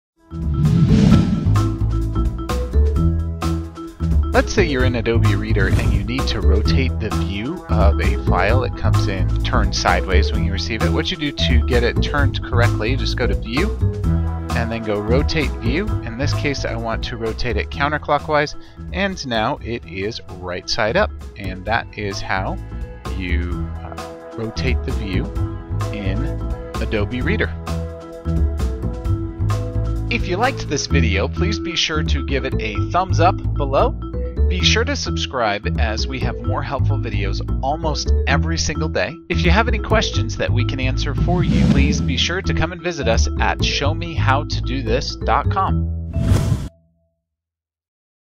How To Rotate View in Adobe PDF Reader
If you get a PDF that is rotated the wrong direction, here's how you rotate it to the right view in Adobe Reader.